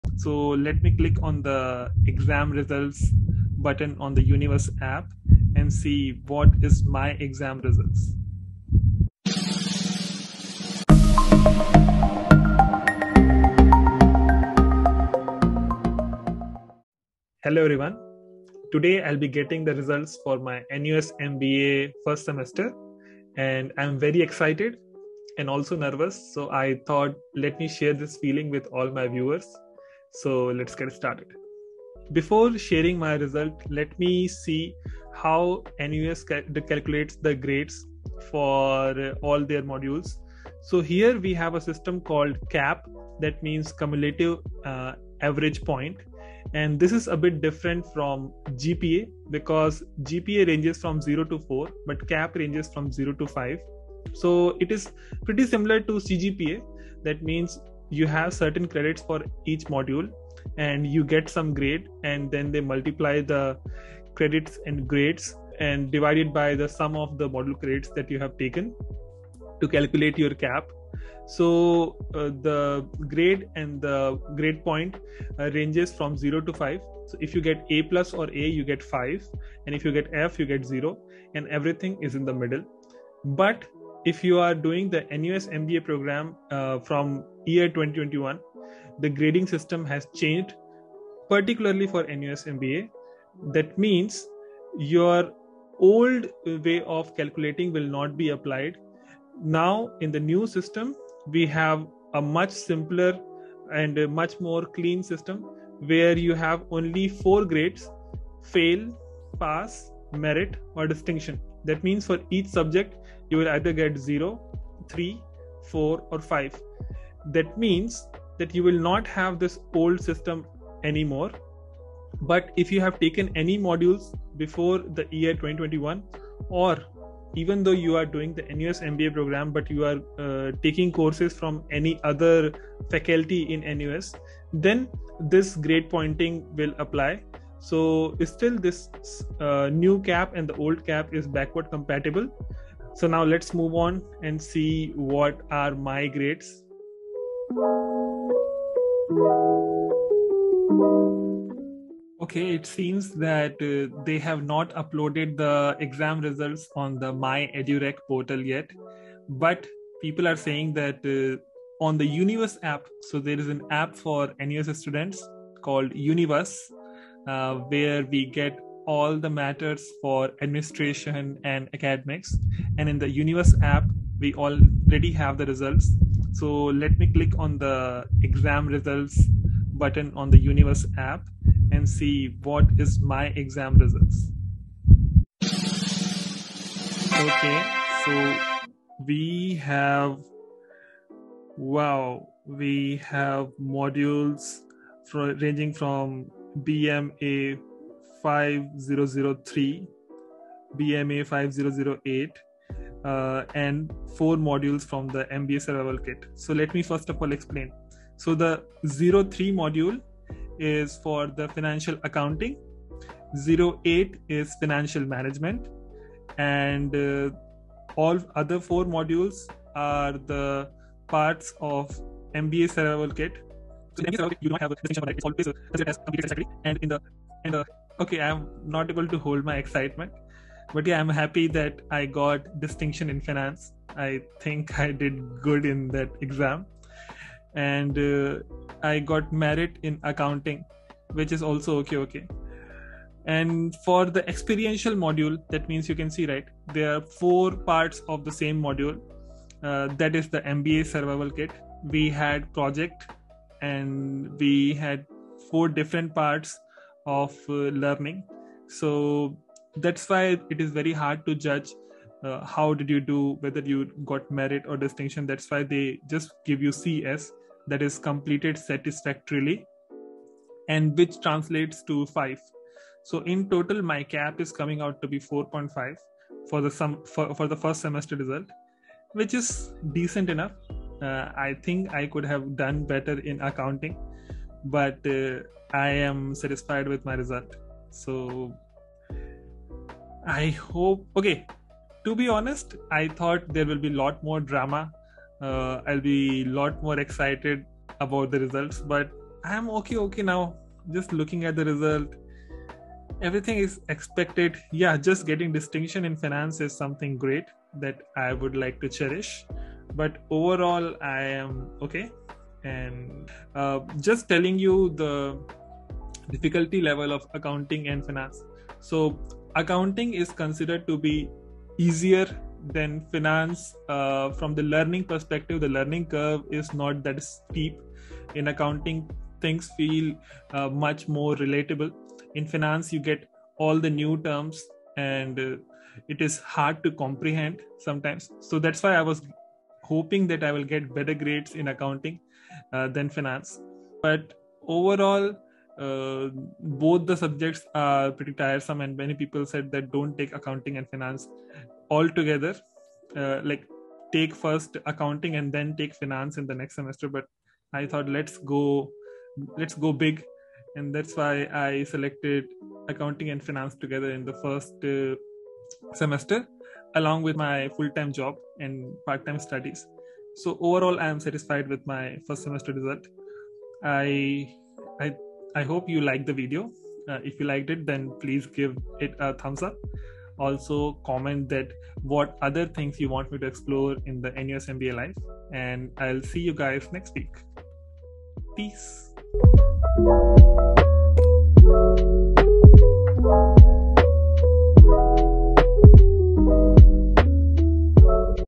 Reacting to my NUS-MBA Sem 1 results | NUS CAP | New Grading System for MBA | Education in Singapore
In this video I'll be reacting to my NUS-MBA Semester 1 results and explain you how NUS MBA CAP grading system works. My first semester modules are as follows
* Accounting
* Finance
* MBA Survival Kit